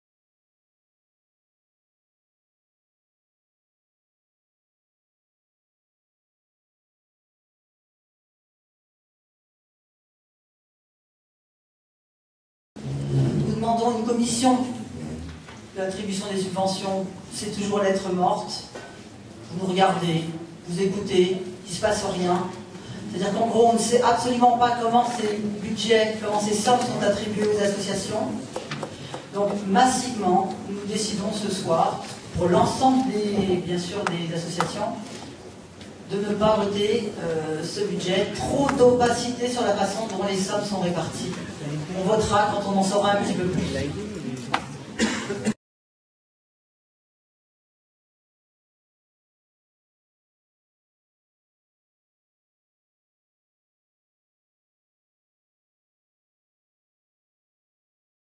Christiane Pesci, EELV, dénonce l'opacité des attributions de subventions par le maire à Bagnolet
Cette intervention s'est déroulé lors du Conseil Municipal de Bagnolet le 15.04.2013 dont l'objet principal était le vote du budget communal 2013. Ce budget a fait l'objet de nombreuses critiques notamment sur le fait que le Maire actuel Marc Everbecq, réalisait un autofinancement en sabrant les budgets des services municipaux et en ne communiquant pas un certains nombre d'information clés pour mieux appréhender le budget. Ici, Christiane Pesci, élue EELV, dénonce l'opacité des attribution de subventions aux associations de Bagnolet.